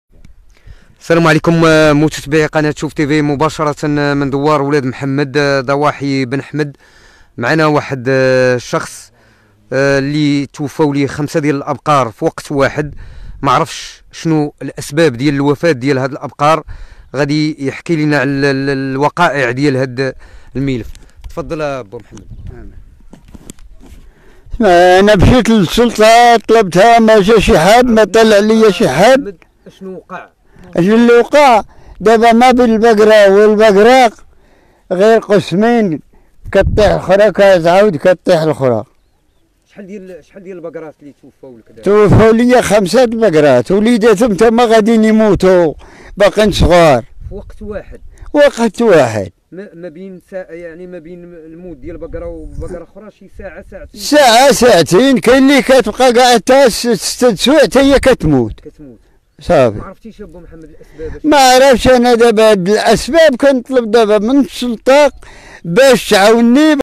شخص ماتوا ليه 5 الأبقار ديالو بطريقة غريبة ولحد الساعة ماعرفش السبب ... شوفو الصدمة ديالو
مباشرة من ضواحي ابن أحمد : شخص ماتوا ليه 5 الأبقار ديالو بطريقة غريبة ولحد الساعة ماعرفش السبب ... شوفو الصدمة ديالو